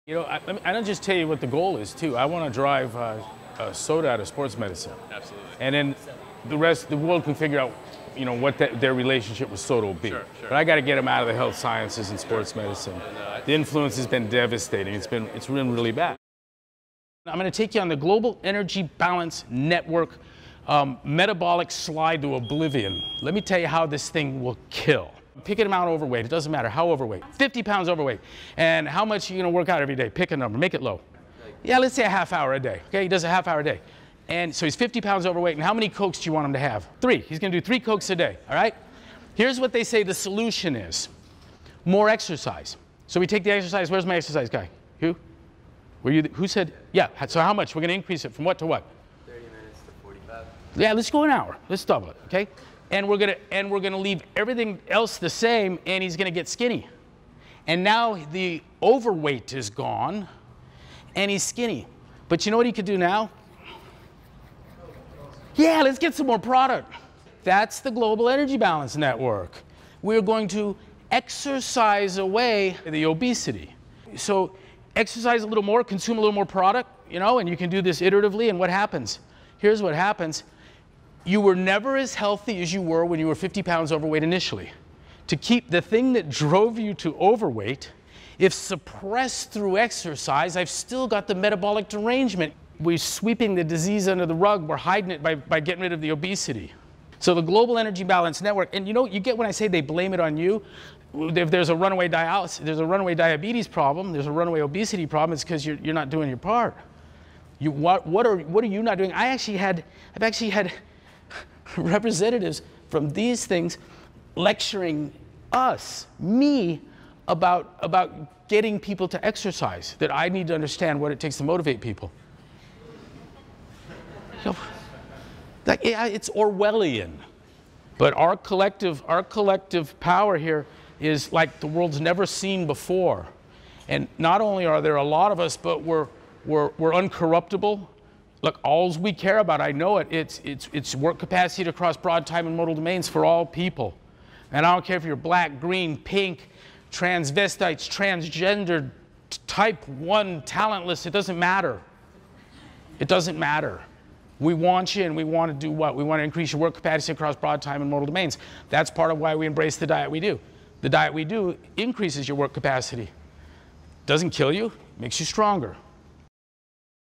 Greg Glassman on "The Global Energy Balance Network metabolic slide to oblivion"
Coca-Cola has been supporting and donating money to the Global Energy Balance Network.

The GEBN promotes "energy balance" as a constant body weight, not taking into account the quality of one's diet or actual weight.